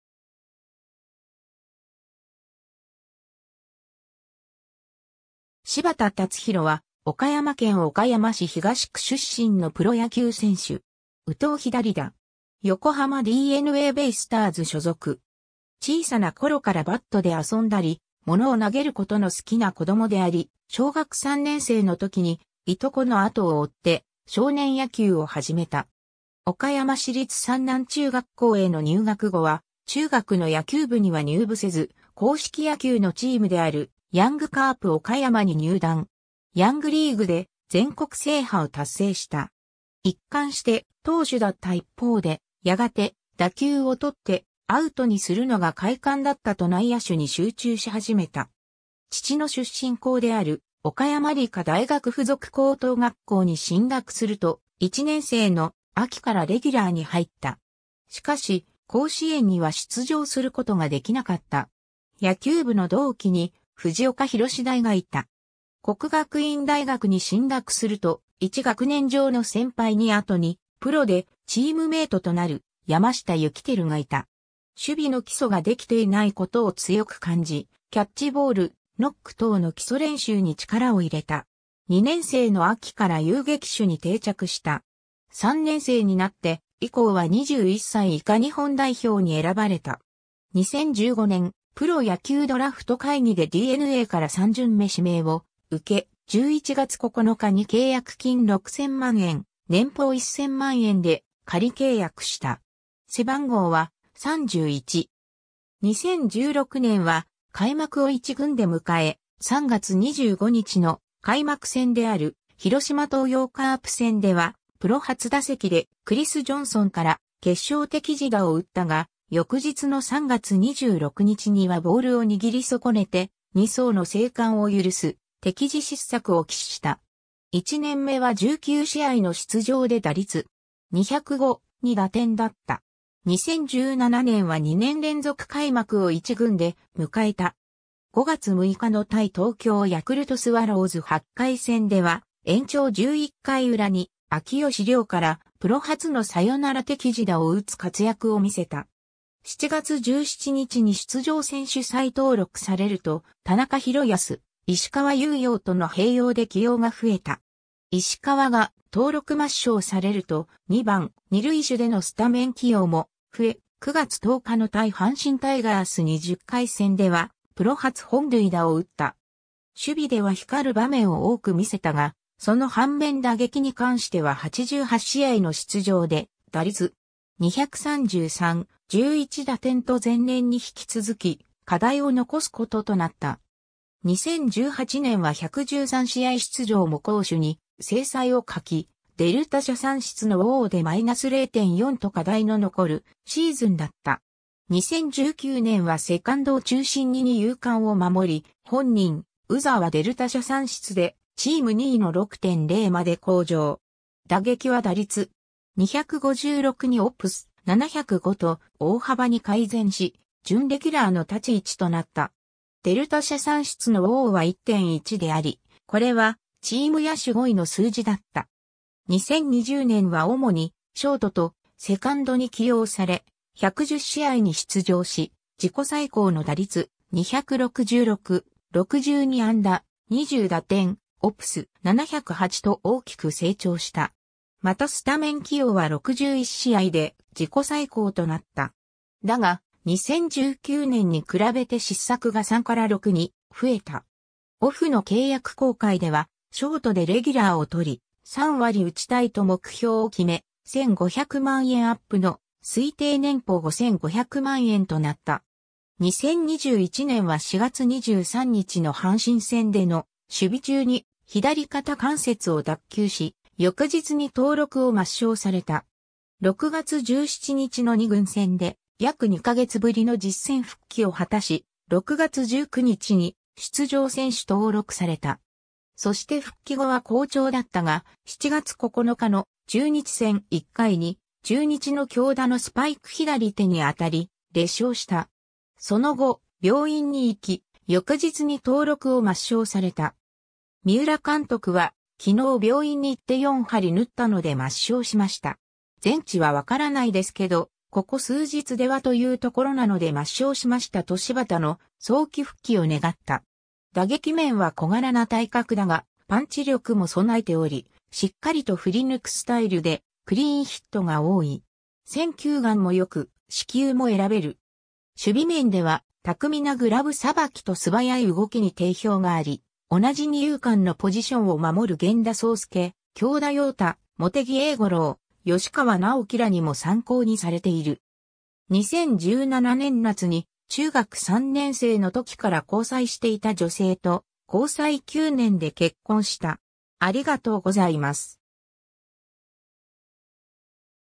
柴田竜拓
柴田 竜拓（しばた たつひろ、1993年12月16日 - ）は、岡山県岡山市東区出身のプロ野球選手（内野手）。
右投左打。
横浜DeNAベイスターズ所属。
 小さな頃からバットで遊んだり、物を投げることの好きな子どもであり、小学3年生の時に従兄弟の後を追って少年野球を始めた。
岡山市立山南中学校への入学後は、中学の野球部には入部せず、硬式野球のチームであるヤングカープ岡山に入団。
ヤングリーグで全国制覇を達成した。
一貫して投手だった一方でやがて、「打球を捕ってアウトにするのが快感だった」と内野手に集中し始めた。
 父の出身校である岡山理科大学附属高等学校に進学すると、1年生の秋からレギュラーに入った。
しかし、甲子園には出場することができなかった。
野球部の同期に藤岡裕大がいた。
 國學院大學に進学すると、1学年上の先輩に後にプロでチームメイトとなる山下幸輝がいた。
守備の基礎ができていないことを強く感じキャッチボール、ノック等の基礎練習に力を入れた。
2年生の秋から遊撃手に定着した。
3年生になって以降は21歳以下日本代表に選ばれた。
 2015年、プロ野球ドラフト会議でDeNAから3巡目指名を受け、11月9日に契約金6,000万円、年俸1,000万円（金額は推定）で仮契約した。
 2016年は開幕を一軍で迎え、3月25日の開幕戦である広島東洋カープ戦（MAZDA Zoom-Zoom スタジアム広島）ではプロ初打席でクリス・ジョンソンから決勝適時打を打ったが、翌日の3月26日にはボールを握り損ねて二走の生還を許す適時失策を喫した。
1年目は19試合の出場で打率.205、2打点だった。
 2017年は2年連続開幕を一軍で迎えた。
5月6日の対東京ヤクルトスワローズ8回戦（横浜スタジアム）では、延長11回裏に秋吉亮からプロ初のサヨナラ適時打を打つ活躍を見せた。
7月17日に出場選手再登録されると田中浩康、石川雄洋との併用で起用が増えた。
石川が登録抹消されると「2番・二塁手」でのスタメン起用も増え、9月10日の対阪神タイガース20回戦ではプロ初本塁打を打った。
守備では光る場面を多く見せたがその反面打撃に関しては88試合の出場で打率.233、11打点と前年に引き続き課題を残すこととなった。
 2018年は113試合出場も攻守に精彩を欠き、DELTA社算出のWARで-0.4と課題の残るシーズンだった。
 2019年はセカンドを中心に二遊間を守り、本人UZRはDELTA社算出でチーム2位の6.0まで向上。
打撃は打率.256にOPS.705と大幅に改善し準レギュラーの立ち位置となった。
DELTA社算出のWARは1.1であり、これはチーム野手5位の数字だった。
 2020年は主にショートとセカンドに起用され、110試合に出場し、自己最高の打率、266、62安打、20打点、OPS.708と大きく成長した。
またスタメン起用は61試合で自己最高となった。
だが、2019年に比べて失策が3から6に増えた。
オフの契約更改では、「ショートでレギュラーを取り、3割打ちたい」と目標を決め、1,500万円アップの推定年俸5,500万円となった。
 2021年は4月23日の阪神戦での守備中に左肩関節を脱臼し、翌日に登録を抹消された。
6月17日の二軍戦で約2ヶ月ぶりの実戦復帰を果たし、6月19日に出場選手登録された。
そして復帰後は好調だったが、7月9日の中日戦1回に中日の京田のスパイク左手に当たり、裂傷した。
その後、病院にいき、翌日に登録を抹消された。
三浦監督は、「昨日病院に行って４針縫ったので抹消しました。全治は分からないですけど、ここ数日では（復帰できない）というところなので抹消しました」と柴田の早期復帰を願った。
 打撃面は小柄な体格だがパンチ力も備えており、しっかりと振り抜くスタイルでクリーンヒットが多い。
選球眼も良く四球も選べる。
 守備面では巧みなグラブ捌きと素早い動きに定評があり、同じ二遊間のポジションを守る源田壮亮、京田陽太、茂木栄五郎、吉川尚輝らにも参考にされている。
 2017年夏に、中学3年生の時から交際していた女性と交際9年で結婚した。